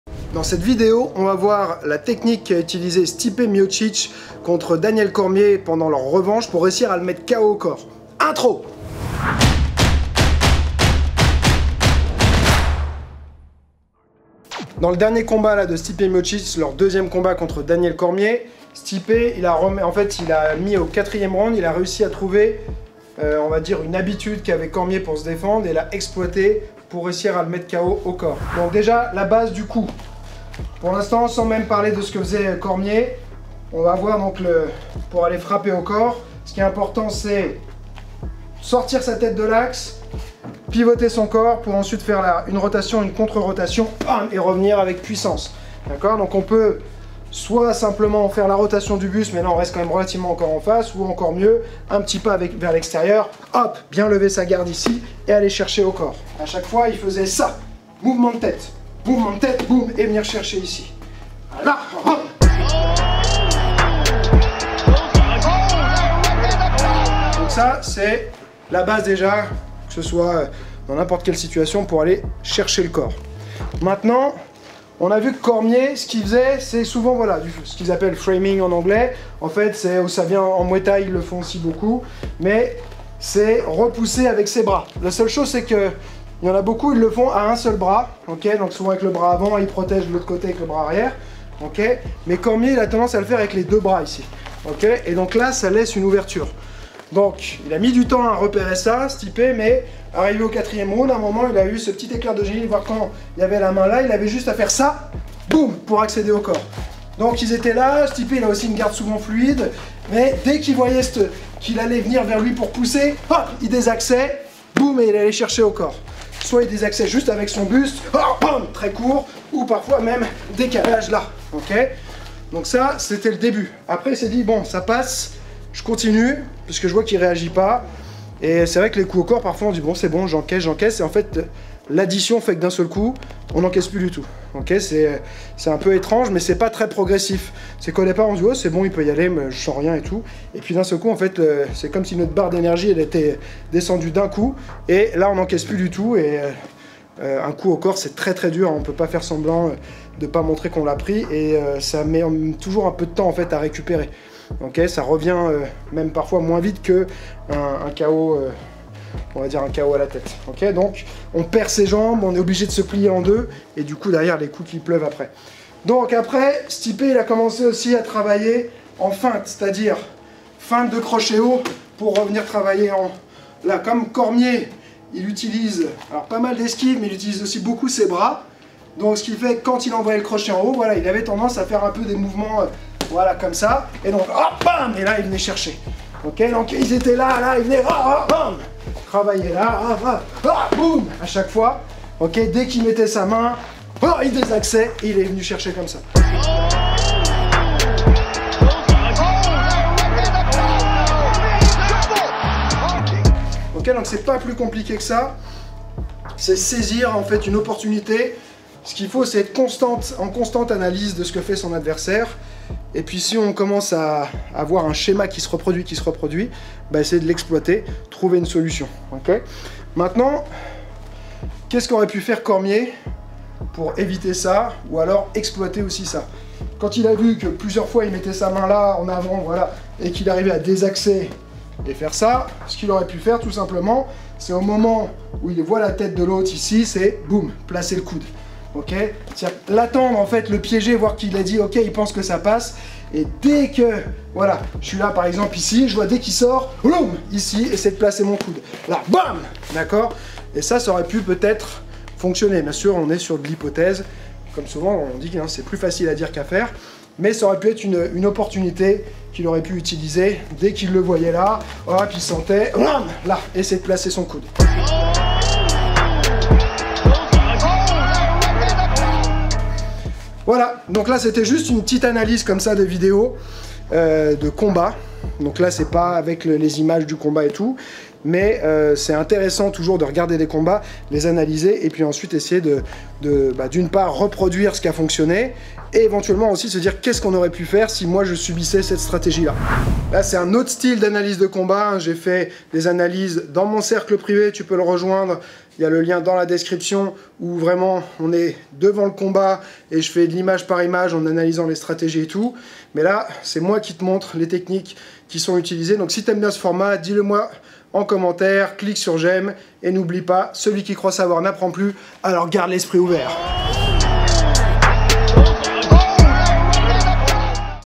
TUTO MMA : COMMENT STIPE MIOCIC A MIS KO DANIEL CORMIER ?
TUTO BOXE : COMMENT STIPE MIOCIC A MIS KO DANIEL CORMIER ?

Je poursuis ce nouveau format d'analyse qui consiste à extraire une technique efficace lors d'un combat afin de la reproduire et de trouver les alternatives qui auraient pu être employées si l'on subissait cette stratégie-là.

Dans cette vidéo je reviens sur le combat entre Stipe Miocic et Daniel Cormier lors de l'UFC 241. Miocic a pris sa revanche le 17 août dernier et a regagné son titre de champion du monde des poids lourds !

En analysant ce combat, je me suis aperçu de la stratégie mise en place par Stipe Miocic au fur et à mesure des rounds et je te l'explique dans cette vidéo !

💥 QUESTION DU JOUR :

 Aimes-tu ce nouveau format d'analyse de combat ?

💎 Si tu veux progresser depuis chez toi, visite ma boutique en ligne où tu retrouveras toutes mes vidéos :